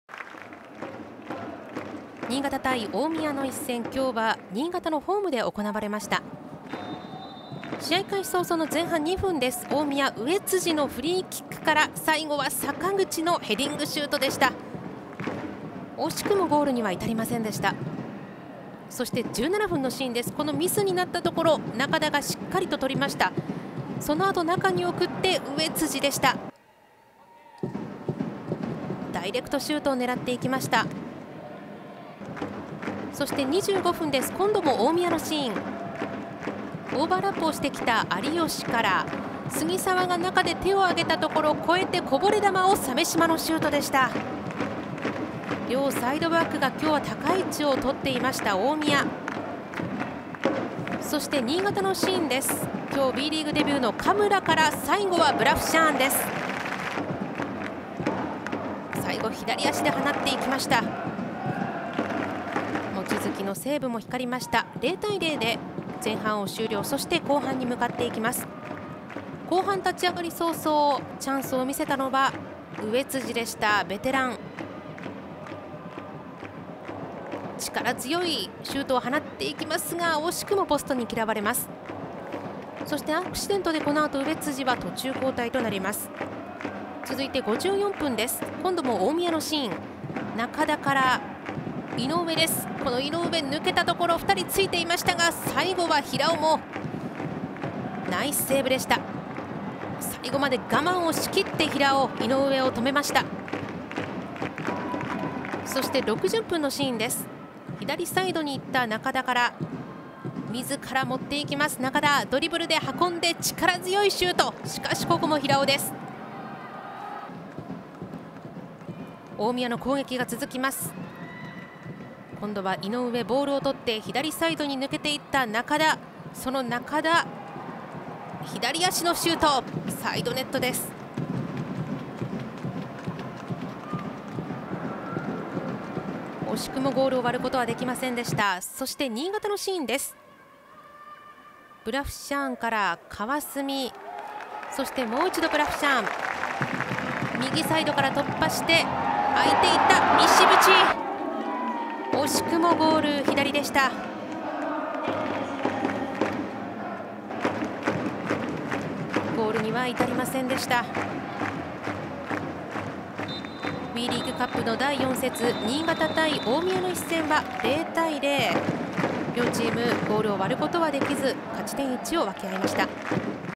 【公式】ハイライト：アルビレックス新潟レディース vs 大宮アルディージャVENTUS【WEリーグカップ GS 第4節 2023.9.17】
【WEリーグカップ GS 第4節 ハイライト動画】

2023年9月17日（日）16時キックオフ
会場：デンカビッグスワンスタジアム